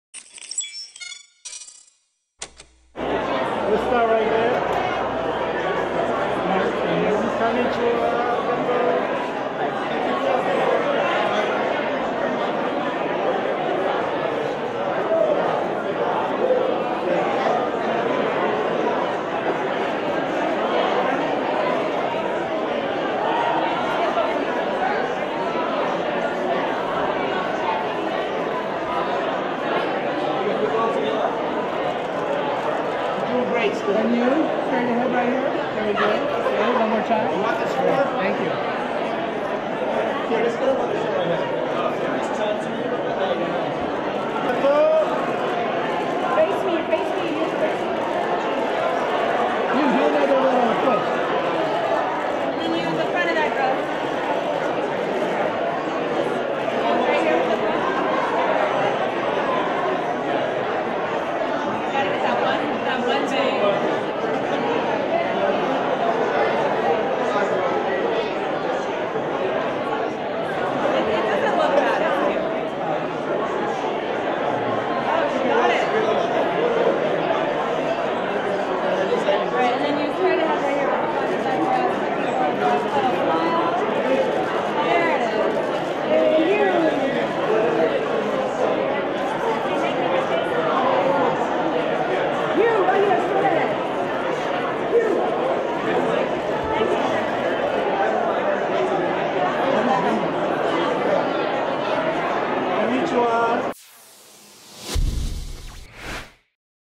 Yu Yamada “Friendly House 30th Annual Awards Luncheon” Red Carpet
Broll footage: Yu Yamada on the red carpet at the Peggy Albrecht Friendly House 30th Annual Awards Luncheon event held at the Beverly Hilton Hotel in Beverly Hills, California USA on October 26, 2019